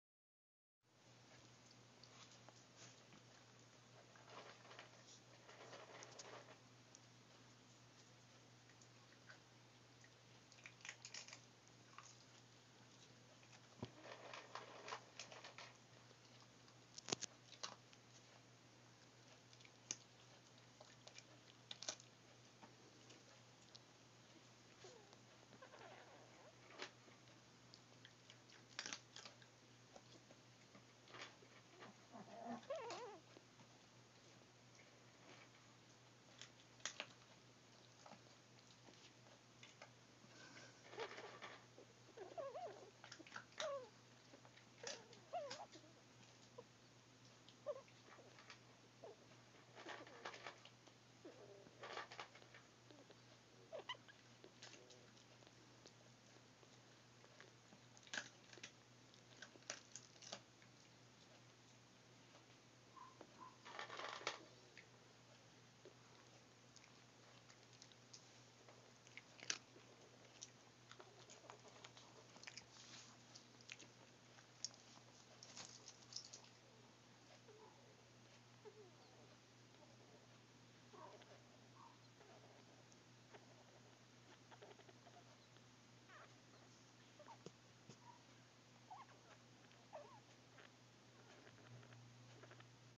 new born chihuahua pups
this isnt art, but i was bored haha

my moms dog recently had puppies and they are going on 4 weeks already! 2 of em are sleeping in this vid but one of them was nawing on his arm/paw before i took this video! haha enjoy :)